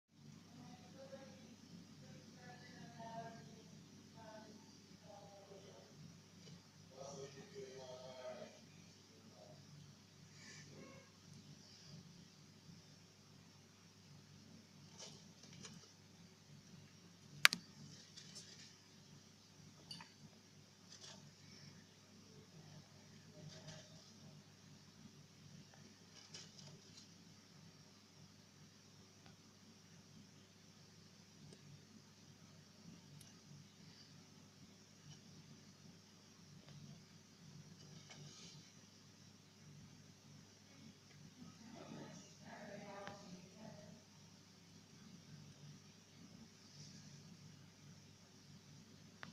Leopard gecko eating Repashy grub pie / Gekončík nočný žerie Repashy grub pie
First time feeding my leos with Repashy grub pie.
Prvé kŕmenie gekončíkov s Repashy grub pie.

Timo (♂) - Super Hypo Tangerine Carrot Tail Baldy